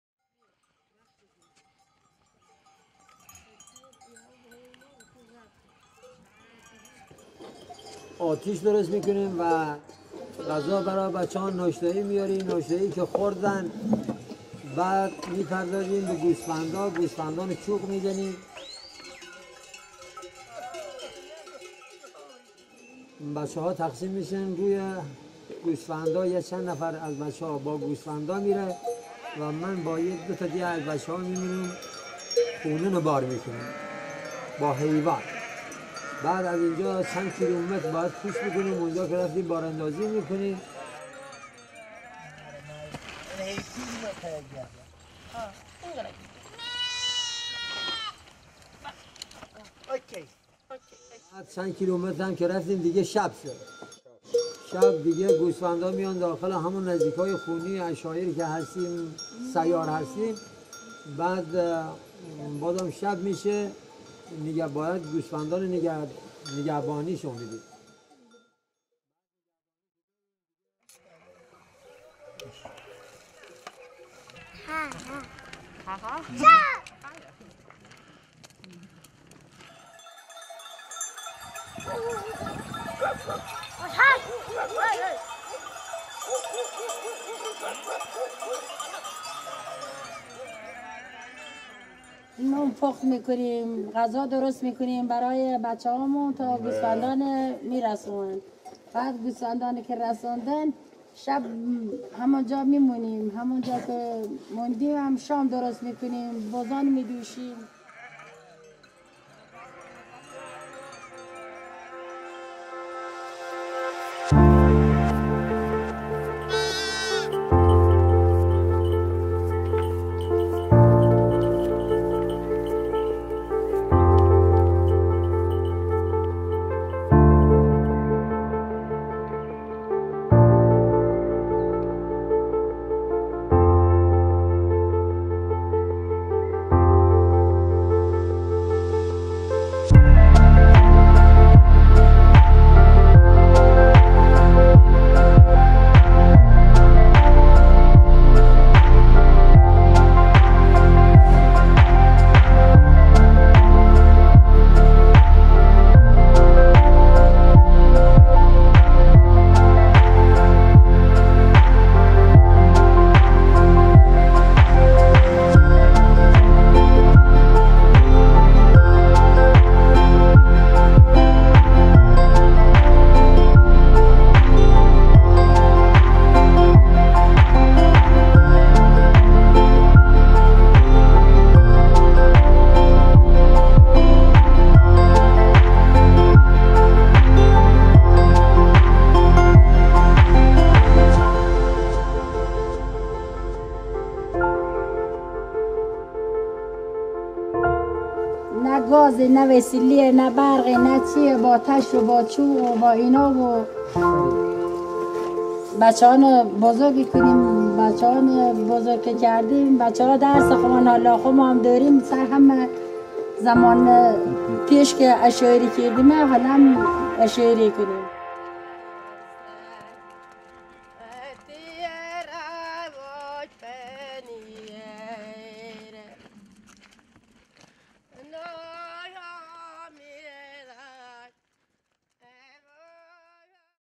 Migration with Bakhtiari Nomads | Kooch | Nomadic lifestyle
Do you know anything about the seasonal migration of nomads?

Persian Bakhtiari nomads are a group of remaining nomad tribes who live in the Zagros Mountains. They are descendants of ancient Bakhtiari nomads. They migrate twice a year to provide their flocks with fresh grass. Once in spring, when they move to their summer pastures, and then in fall when they move to lower altitudes.
In recent years, the number of these authentic people has declined dramatically. Now, we’re trying to help them in preserving their authentic culture, and we hope to save them from their imminent extinction. We believe their unique lifestyle & authentic culture need to be preserved.
The first-hand experience of migrating with Bakhtiari Nomads is far beyond watching any videos or photos.